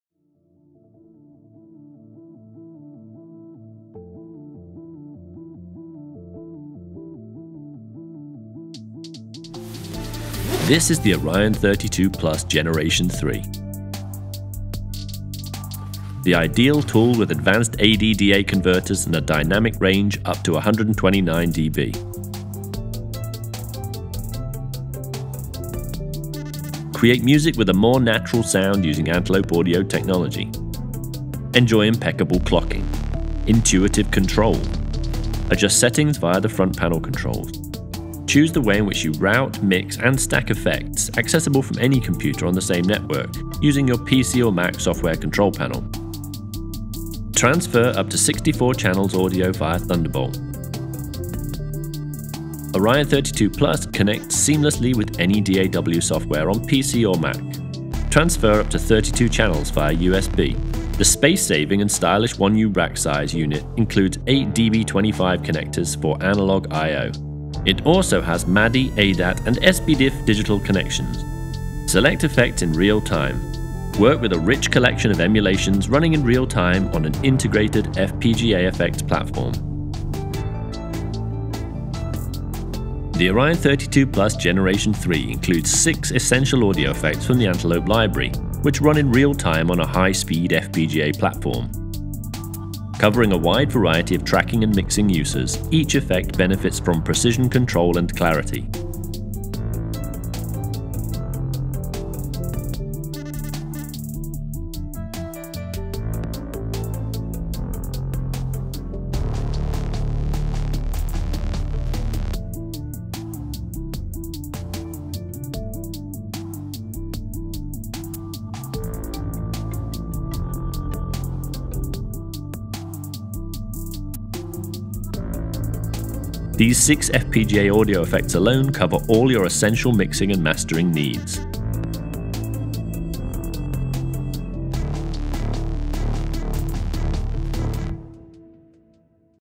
Unboxing Orion32+ Gen 3 | Antelope Audio
Orion32+ Gen 3 is the latest revision of the top selling 64-channel audio interface by Antelope Audio. Take a look inside its box and consider bringing your sound and workflow to the next level by adding Orion32+ Gen 3 to your setup.

With dynamic range enhanced to the stunning 129dB, cutting-edge AD/DA conversion ultra stable Win and Mac drivers for sub-millisecond latency, Orion32+ Gen 3 is the latest incarnation of a unit, which has already become a must for any top-tier recording or post-production facility. Offering seamless connecting with any DAW, Orion32+ | Gen 3 features low-latency recording & playback of up to 64 simultaneous 24/192 audio channels via Thunderbolt™ and up to 32 channels over USB. Coming with a sleek new faceplate, the single rack space interfaces offers 8x DB25 connectors for Analog I/O. Digital connections are 1x MADI, 2x ADAT, and 1x S/PDIF.

Among the interface’s main strengths is a careful selection of hardware-quality real-time effects selected from the growing and authentically modeled Antelope Audio FPGA FX. The six available tools however are chosen in a way to ensure you are equipped with everything you need for both mixing and mastering purposes. Additional plugins can be purchased in case you need your Orion32+ FX library expanded.